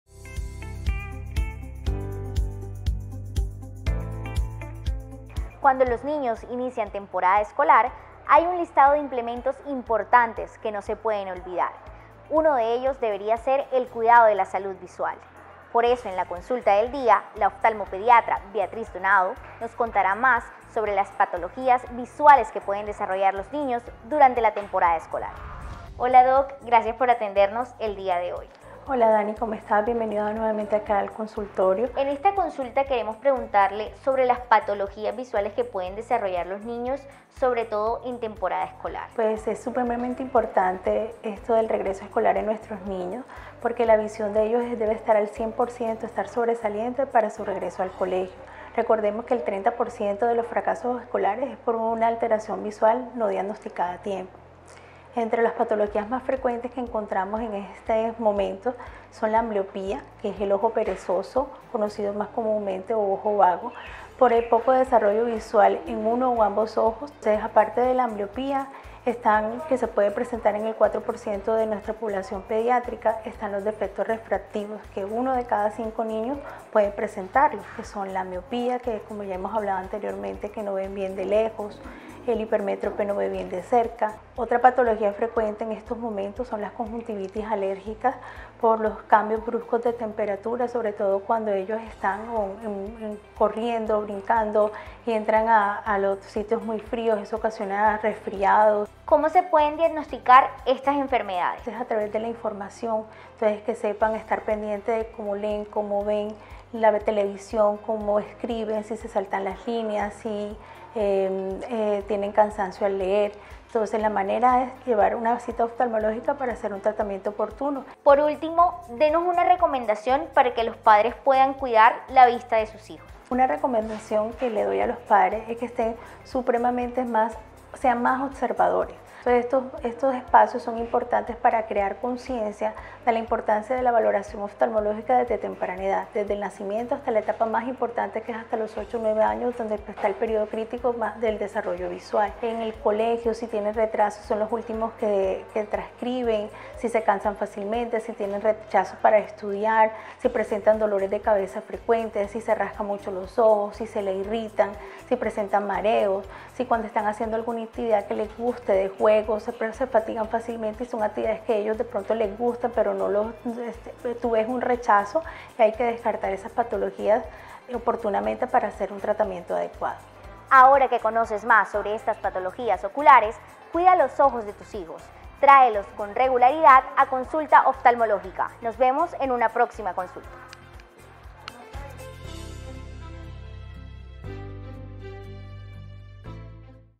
Cuidado visual en niños | Consulta del Oftalmología
En este consulta la oftalmóloga pediatra, Beatriz Donado, nos cuenta las enfermedades o patologías a las que pueden enfrentarse niños y jóvenes durante la temporada escolar.